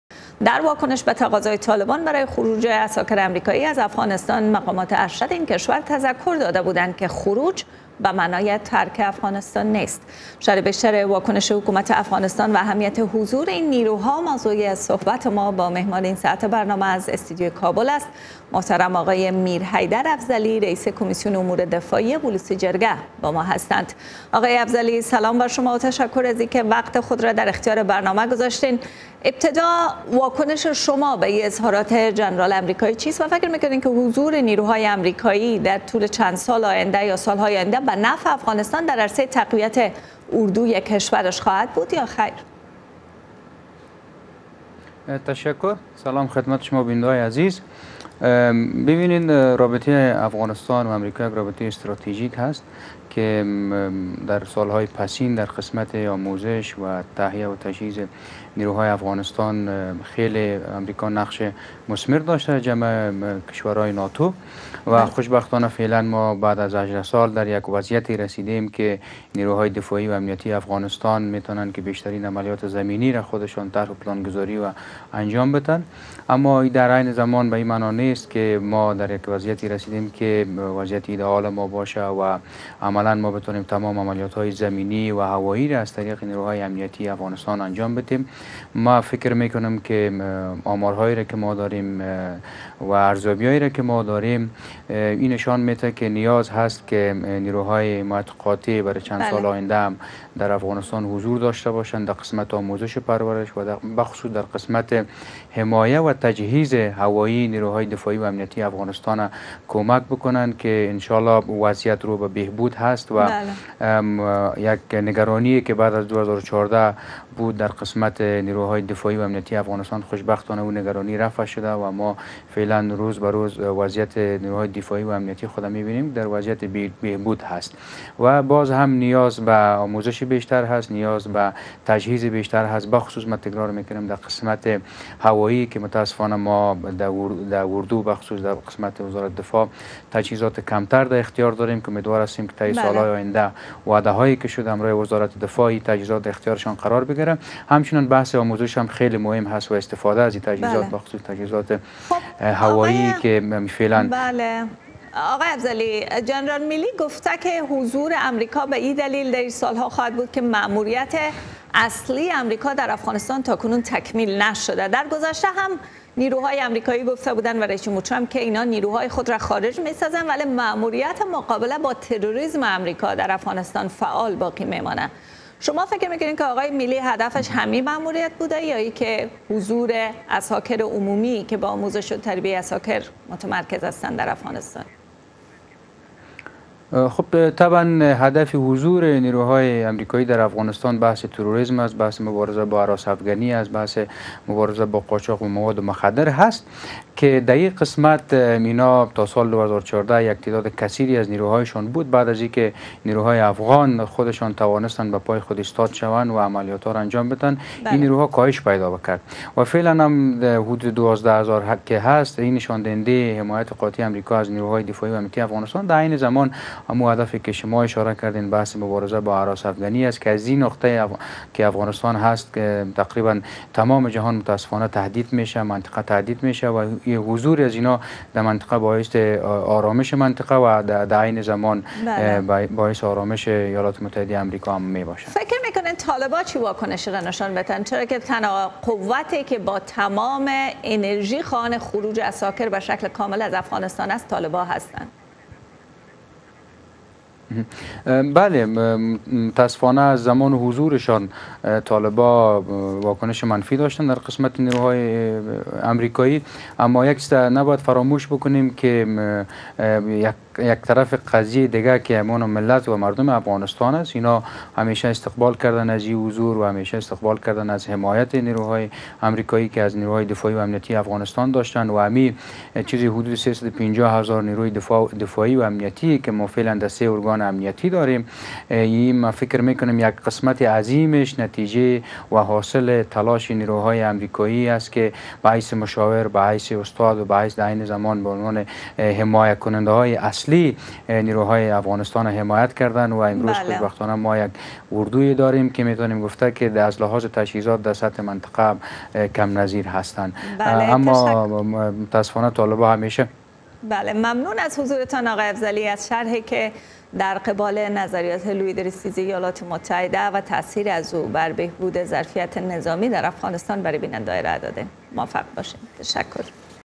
Afghan Government Welcomes General Milley's Comment on Afghanistan
U.S. Army General Mark Milley, chairman of the Joint Chiefs of Staff, has predicted that American troops, already in Afghanistan for 18 years, would remain in the country "for several more years." Afghan government welcomed the decision and said Afghan security forces still need the US and allies support to be able to defend Afghanistan
11/11/19